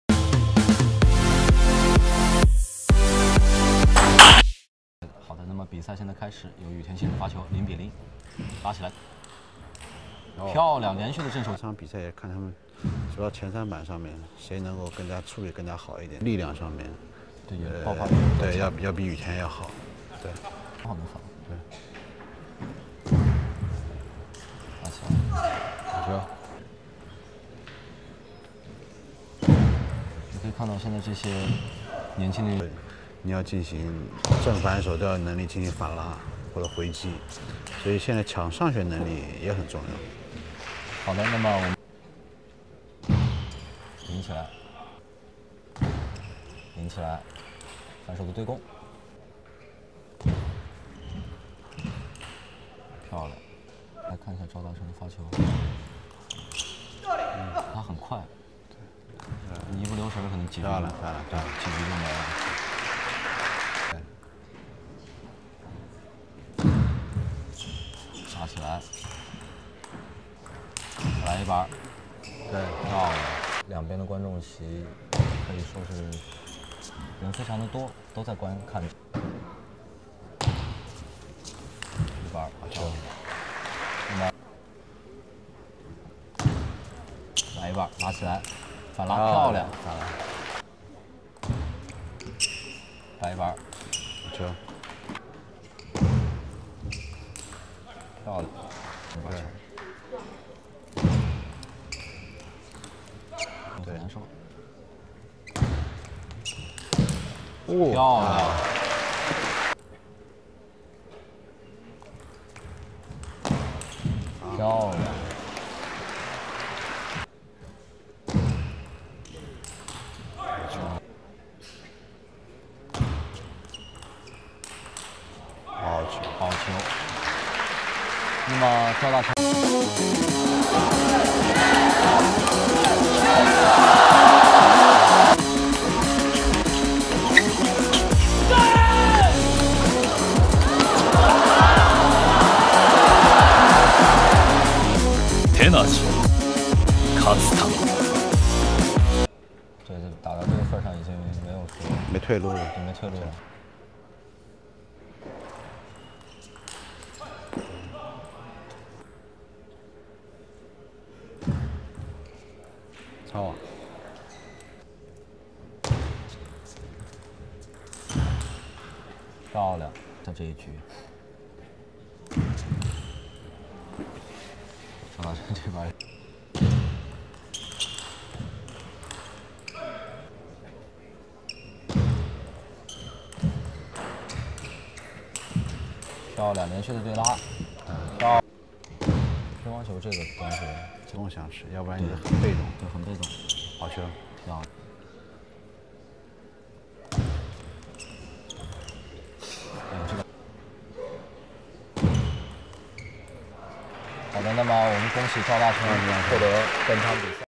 2016 World Cadet Challenge Highlights: Yukiya Uda vs Cho Daeseong (Final)
Review all the highlights from the 2016 World Cadet Challenge Highlights: Yukiya Uda vs Cho Daeseong (Final)